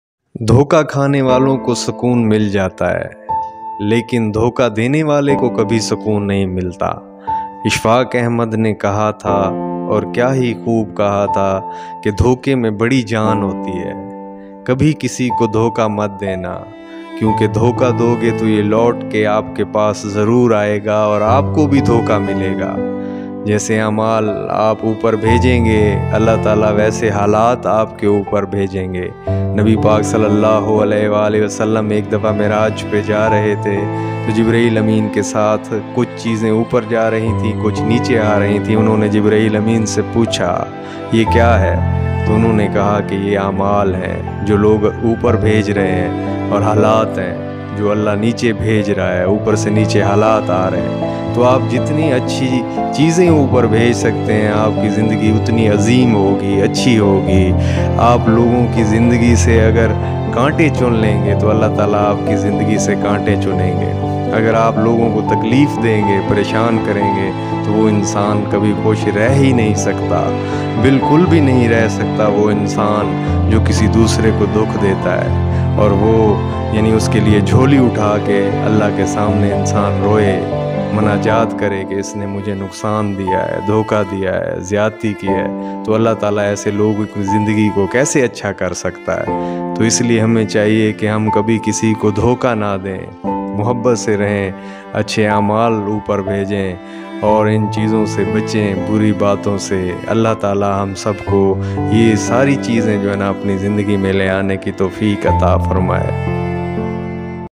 Dhoka دھوکہ Kisi ko dhoka mat dain Khudara
السلام علیکم ویورز ان چھوٹے چھوٹے کویٹس کو ضرور سنے اور ان کے اوپر عمل کرنے کی کوشش کریں اللہ پاک اپ کا حامی و ناصر ہو